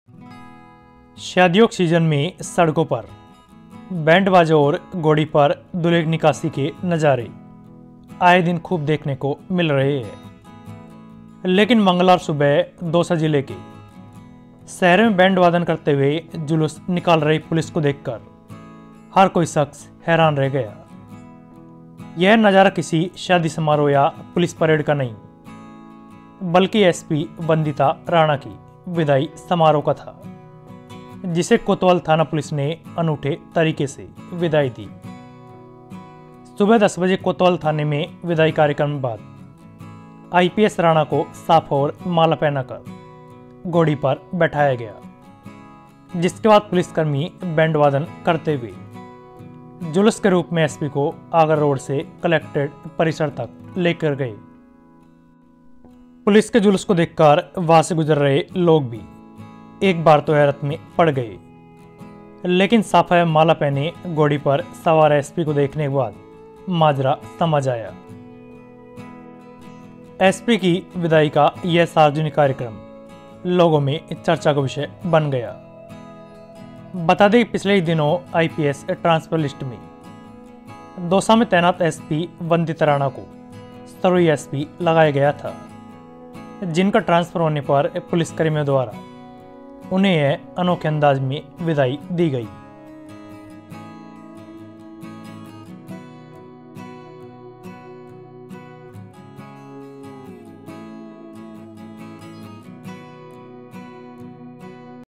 IPS वंदिता राणा​: दौसा में SP को यादगार विदाई, घोड़ी पर बैठाकर 4KM तक गाजे-बाजे से निकाला जुलूस
Dausa News: दौसा एसपी वंदिता राणा का सिरोही पुलिस अधीक्षक पद पर तबादला होने के बाद मंगलवार को गाजेबाजे के साथ घोड़ी पर बिठाकर शहर भ्रमण करवाया गया. साल 2017 बैच की ऑफिसर वंदिता राणा (36) दौसा जिले की 34वीं पुलिस अधीक्षक बनी थी

वही एसपी वंदिता राणा को गाजेबाज़े के साथ घोड़ी पर बिठाकर शहर भ्रमण करवाया गया.  एसपी वन्दिता राणा का नगर भ्रमण कोतवाली से शुरू होकर एसपी ऑफिस पर  जाकर संपन्न हुआ.  जहां वंदिता राणा ने कहा कि, मुझे इतना प्रेम देकर लौटाया कि मैं इतनी भाव विभोर हूं शब्दों में बयां नहीं कर सकती .

गौरतलब है कि, एसपी वंदिता राणा के बाद अब दौसा की नई एसपी के रूप में रंजिता शर्मा को लगाया जाएगा. जो 21फरवरी को कार्यभार ग्रहण करेगी. राजस्थान सरकार ने पुलिस महकमें में बड़ा फेरबदल किया है, इसी क्रम में राजस्थान के 65 IPS अधिकारियों का तबादला भजनलाल सरकार द्वारा किया गया है. आईपीएस तबादलों की सूची में कई जिलों के एसपी में फेरबदल किया